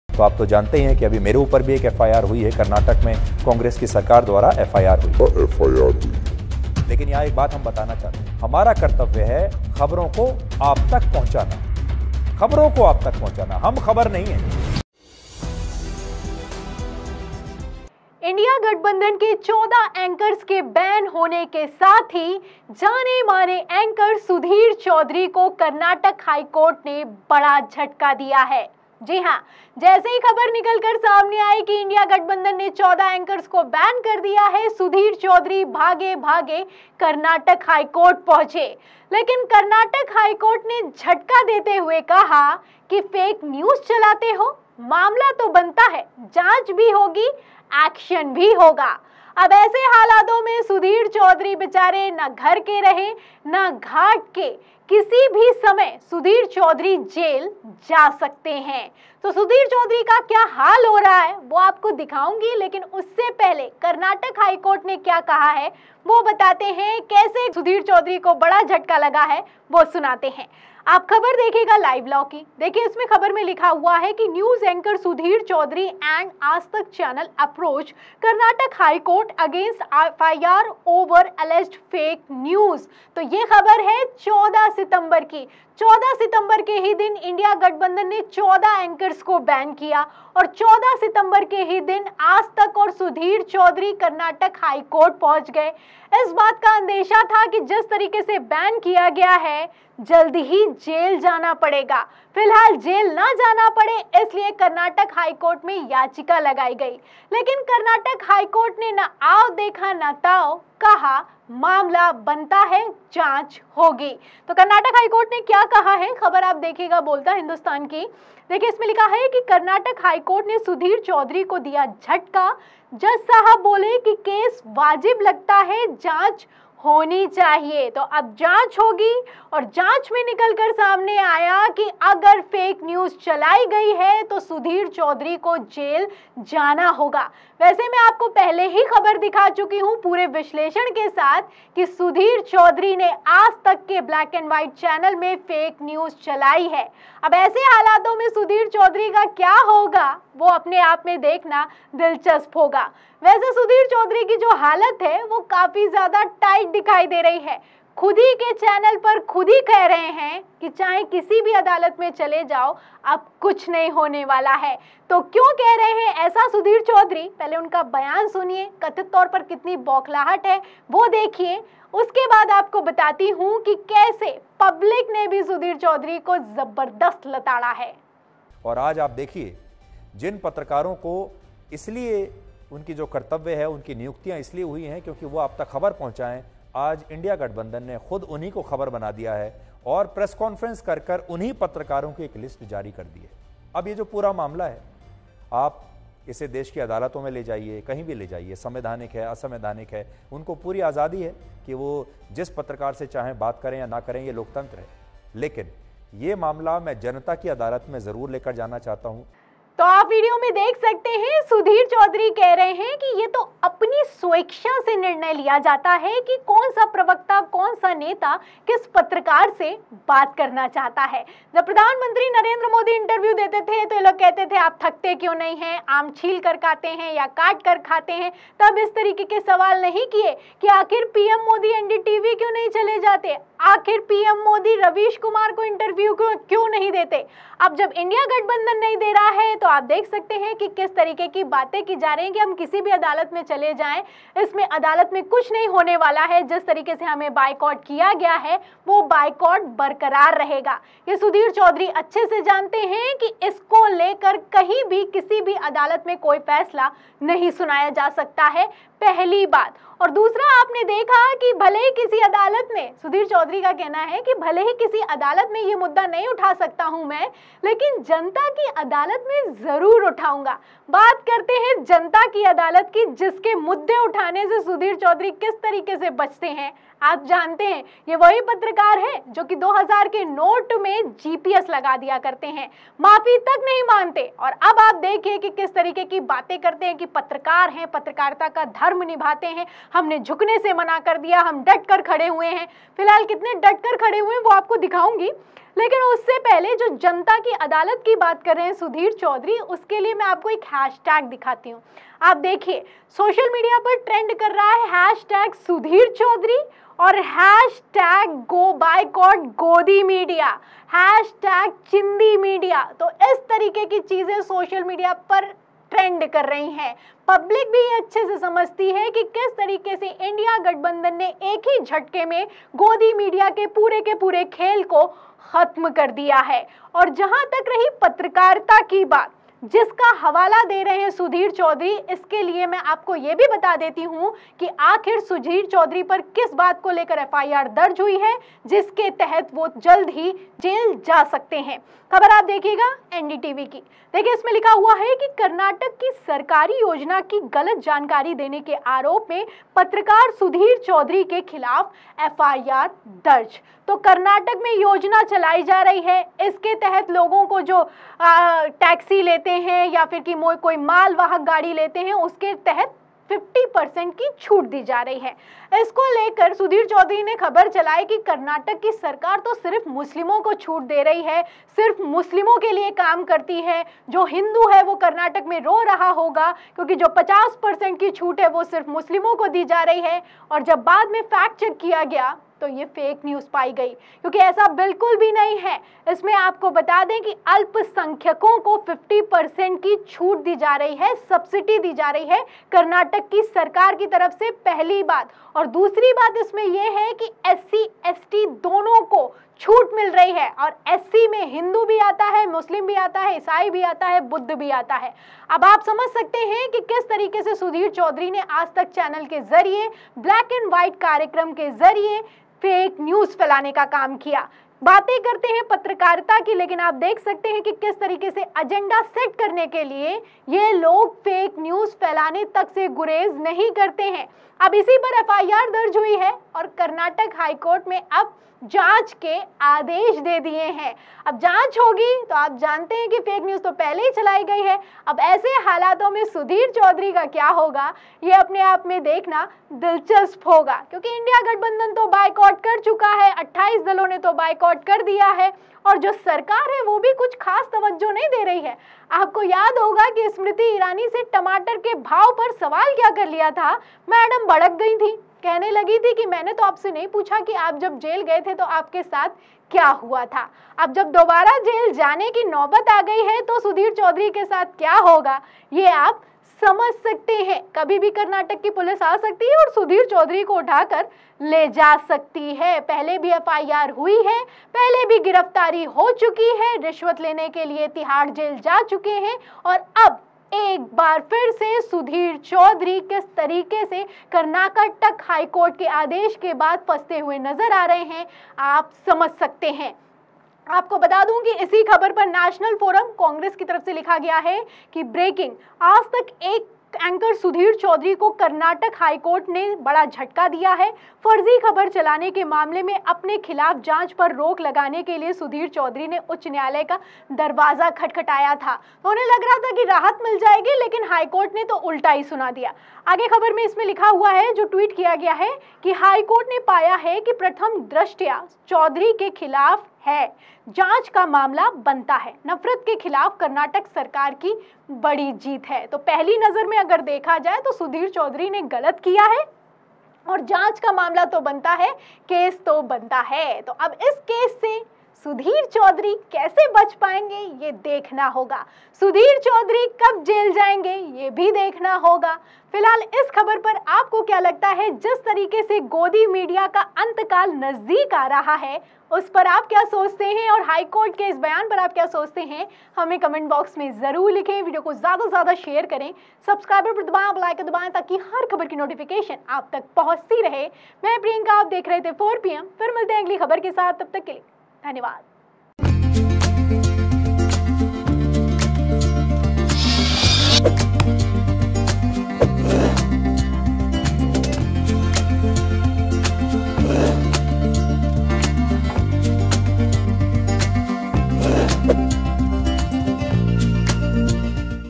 किसी भी वक्त जेल जाएंगे सुधीर चौधरी, पब्लिक ने भी बायकॉट कर दिया!
4PM News Network 🌎